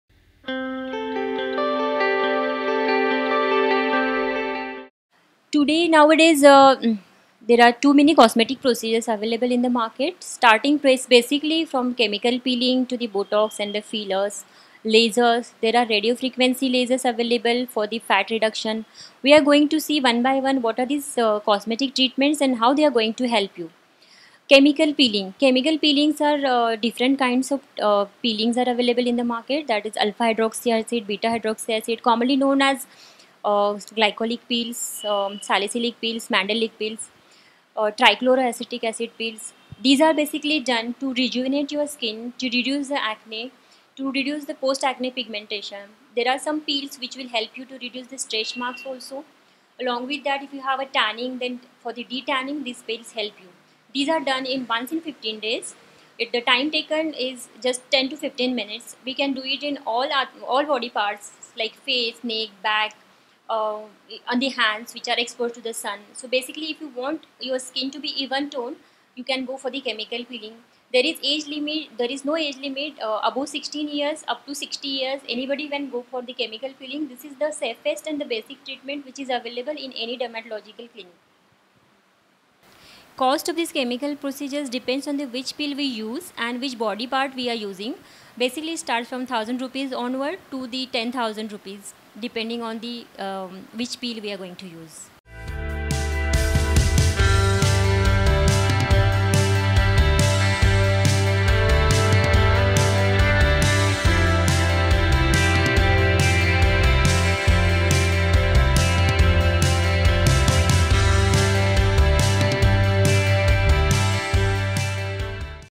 Chemical Peeling - Procedure, Uses and Cost in India
What are the types of cosmetic procedures? What is chemical peeling and what does it cost in India? Dr. Sadhana Jadhavar describes about chemical peeling, a cosmetic procedure performed to rejuvenate skin, reduce acne and also gives information about the cost of chemical peeling in India.